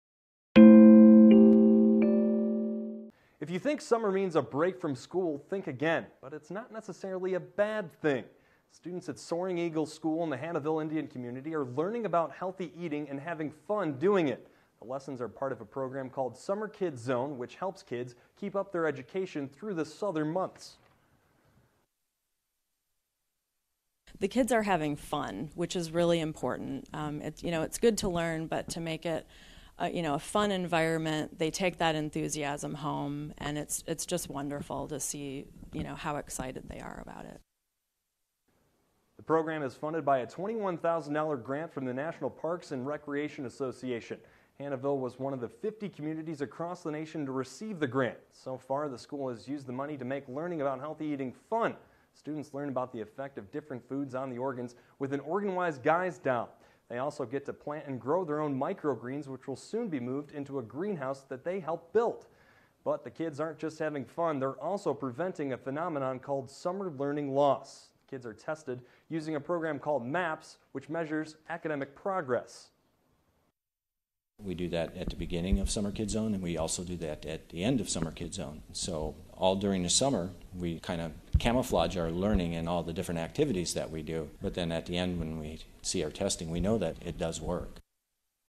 Hannahville Indian Community Makes Summertime Healthy
Thanks to the Walmart Foundation and the National Recreation and Park Association, Hannahville Indian Community received a grant for The OrganWise Guys healthy living materials. They are enjoying creating healthier kids during the summer months.